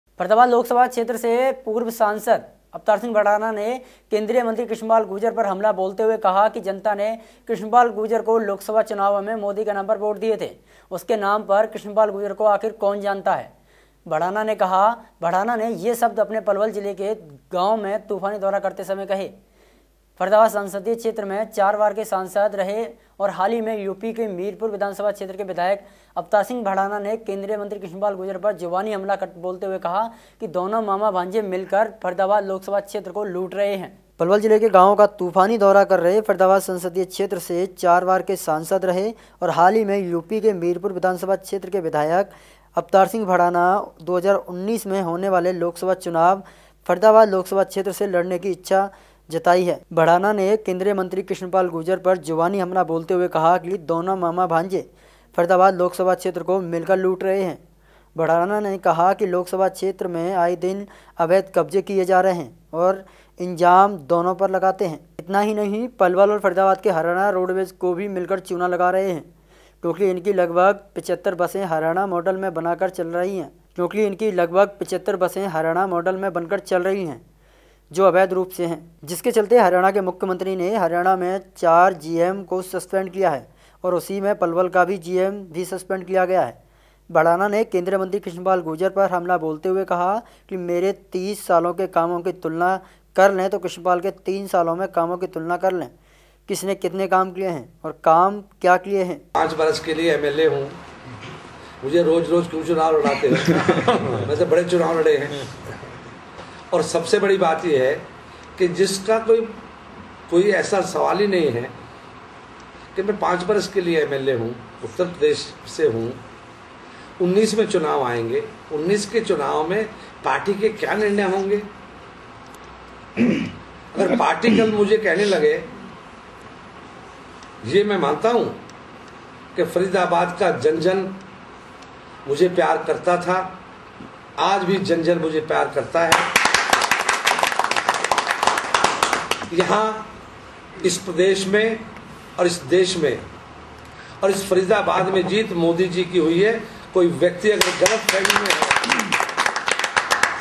पलवल ║केंद्रीयमंत्री कृष्णपाल गुर्जर पर भडाना ने बोला हमला
bvmnews haryananews topnews bvmnewslive latestnews भड़ाना ने केंद्रीय मंत्री कृष्ण पाल गुर्जर पर हमला बोलते हुए कहा की जनता ने कृष्ण पाल को लोक सभा चुनाबों में मोदी के नाम पर वोट दिए थे उसके नाम पर कृष्ण पाल को कौन जनता है ! भड़ाना ने यह सब अपने पलवल जिले के गावों का तूफानी करते समय कहे ! फरीदाबाद संसदीय क्षेत्र से 4 बार के सांसद रहे और हाल में यूपी के मीरापुर विधान सभा क्षेत्र के विधायक अवतार भड़ाना ने केंद्रीय मंत्री कृष्ण पाल गुर्जर पर जुबानी हमला बोलते हुए कहा की दोनों मामा भांजे मिलकर फरीदाबाद लोक सभा क्षेत्र को लूट रहे हैं!